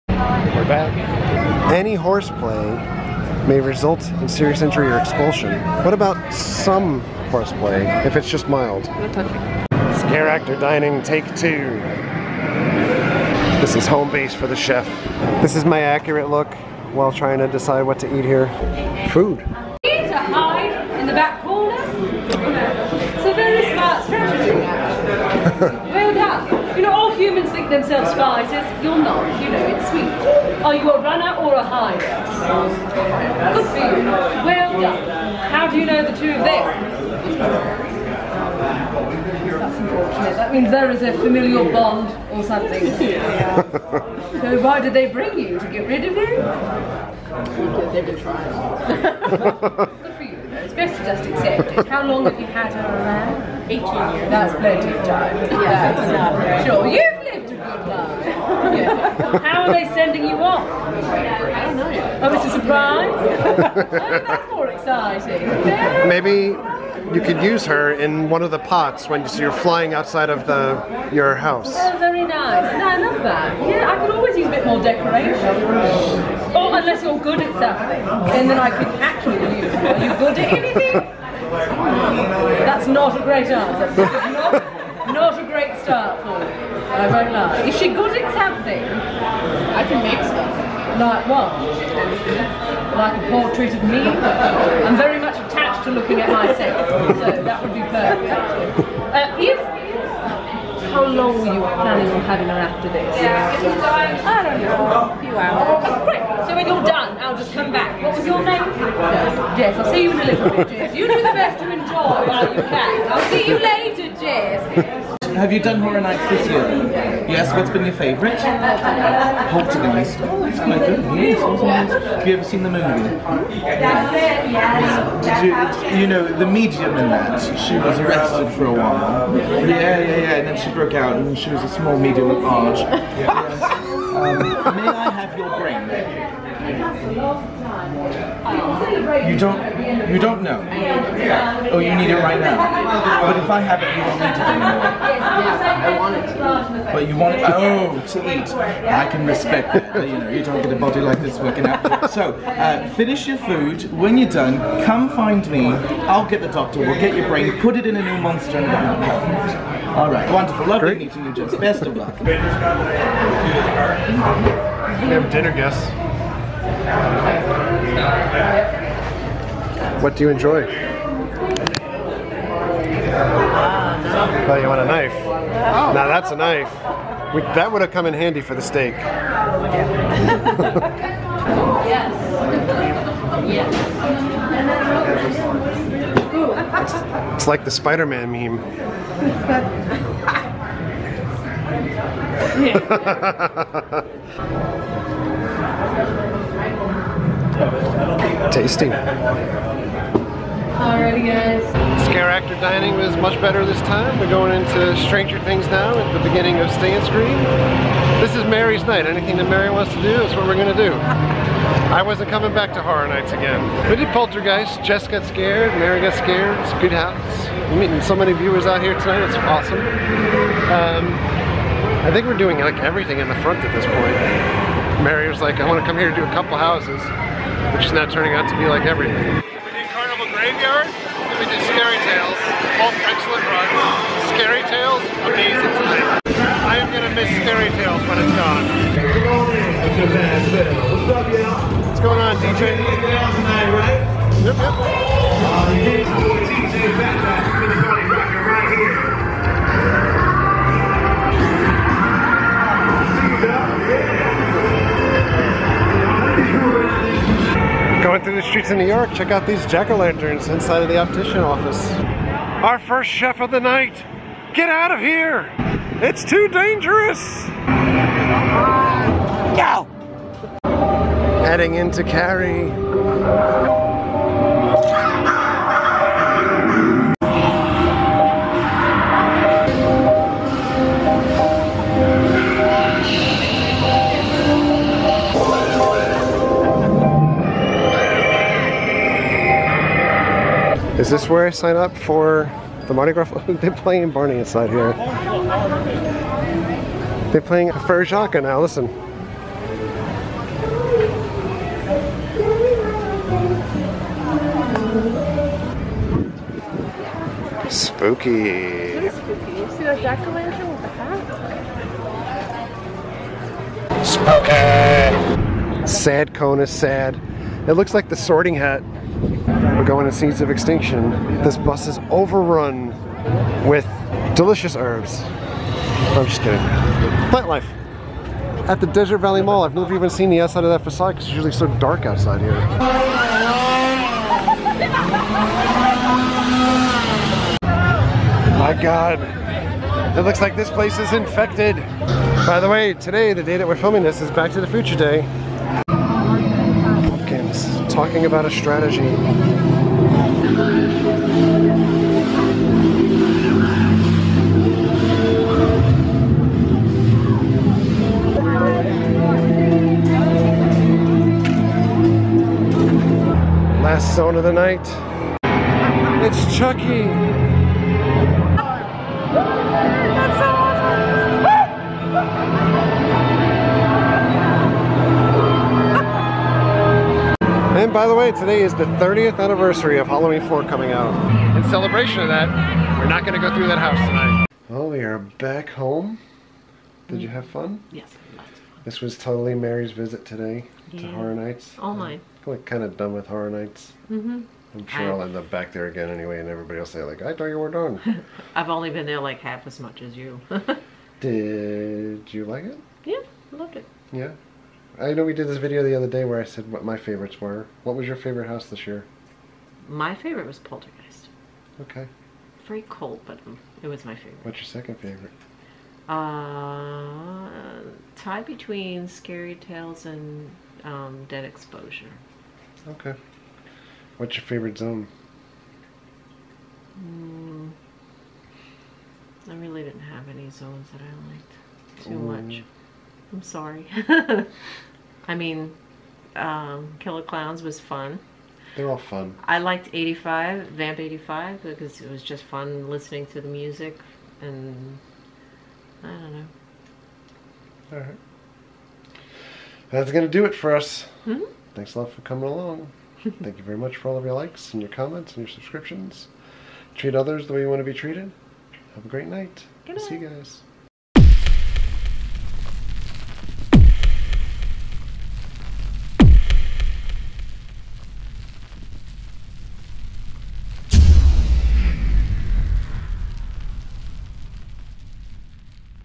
Return To Scareactor Dining & A Super Quick Night Of Universal Orlando Halloween Horror Nights 28
We returned to Universal Orlando for their Halloween Horror Nights Scareactor Dining. We had a much better time this round with plenty of character interactions. The rest of the night was spent on a mad dash to avoid crowds while visiting all of the areas that Mary wanted to see.

Watch the show! Wear the shirt! Shirts are available through Spreadshirt.

View From The Cheap Seats